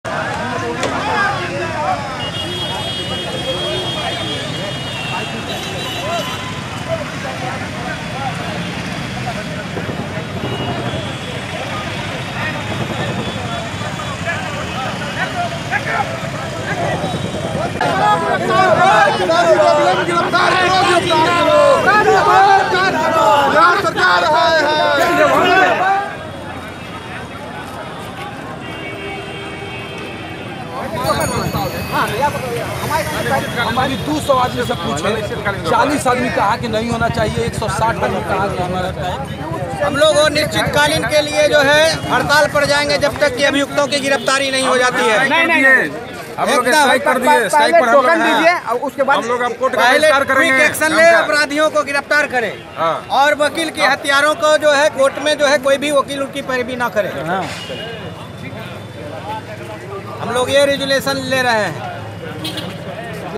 हत्या के विरोध में सड़क पर उतरे हाईकोर्ट के वकील, पुलिस के साथ झड़प
पटनाः बेखौफ अपराधियों ने बुधवार की सुबह पटना हाईकोर्ट के वकील जितेंद्र कुमार की गोली मारकर हत्या कर दी. इस मामले में पटना हाईकोर्ट के वकीलों ने बेली रोड हत्या के विरोध में उग्र प्रदर्शन किया और अपराधियों की तत्काल गिरफ्तारी की मांग की.